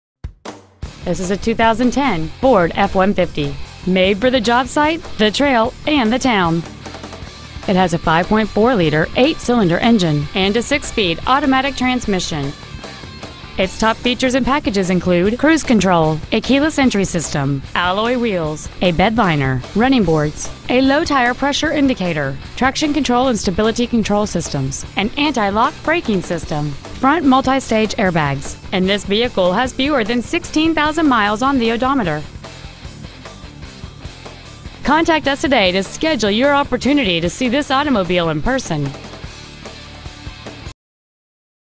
2010 Ford F150 SuperCrew Lubbock TX
We are proud to present this 2010 Ford F150 SuperCrew .

 We've been honored to serve the Amarillo TX area , we promise that your experience at our dealership will exceed your expectations !

 Year : 2010

 Make : Ford

 Model : F150 SuperCrew

 Engine : V8 Flex Fuel 5.4L

 Trans . : 6-Spd Automatic

 Exterior : Blue

 Miles : 15,286

 Interior : Gray

 Bobby Duby Motors

 4215 Georgia

 2010 Ford F150 SuperCrew Amarillo TX